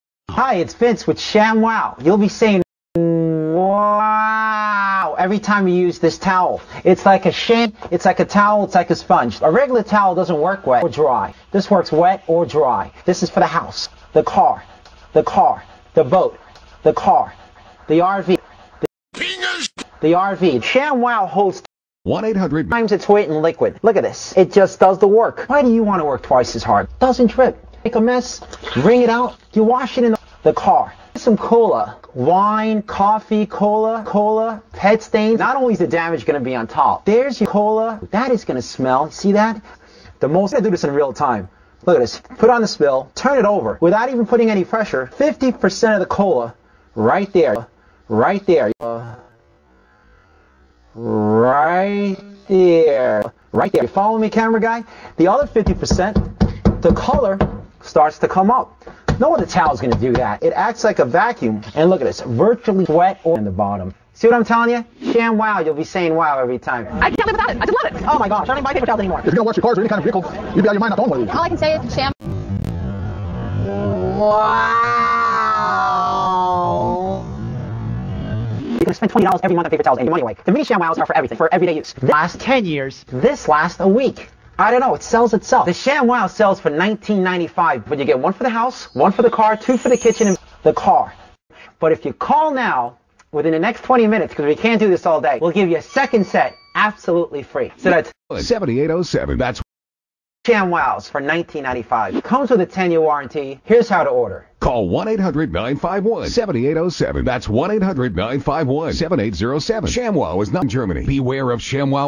YouTube Poop: It's a Sham, Wow!
My very first effort in the art of YouTube Poop making...

Please don't be too harsh if it sucks. It's my first poop, and I made it with Windows Movie Maker, so consider those two things before you say it sucks. I wanted to do A LOT more with this, but due to my lack of experience and the fact that Windows Movie Maker sucks, I couldn't. Maybe if I ever get a better software and more experience, you'll get to see this exactly how I wanted it to be.

Also, the ending was supposed to say "Beware of ShamWow", but it got cut off -_-

Update 1/29/09 - Wow! Over 3,500 views! That's 3,500 more than I ever thought this video would get!